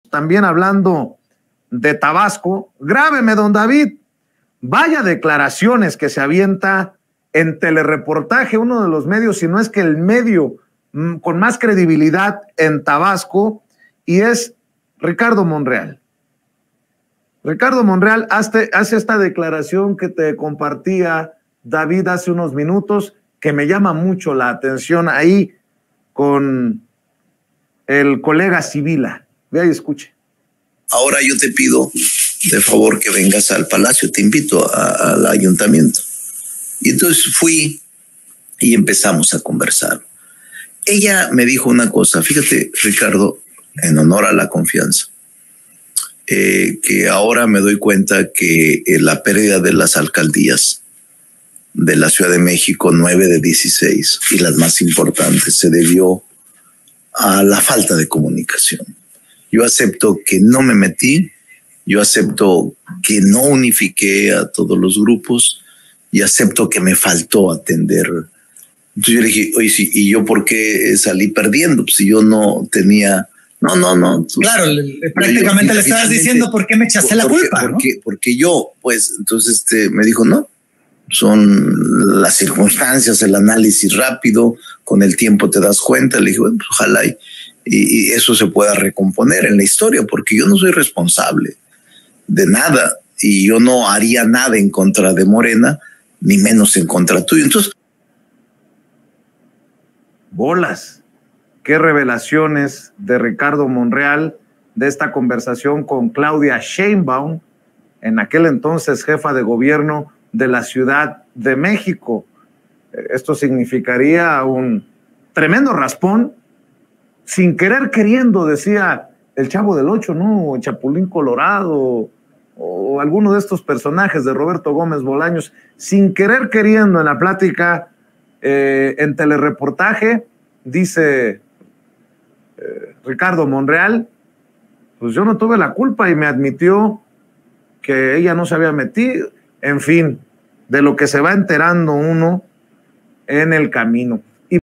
DE LO QUE SE VA ENTERANDO UNO… MONREAL, SIN QUERER QUERIENDO, HACE REVELACIONES SOBRE SHEINBAUM
Ricardo Monreal hizo una serie de revelaciones, sin querer queriendo, sobre Claudia Sheinbaum durante una entrevista… De lo que se va enterando uno en el camino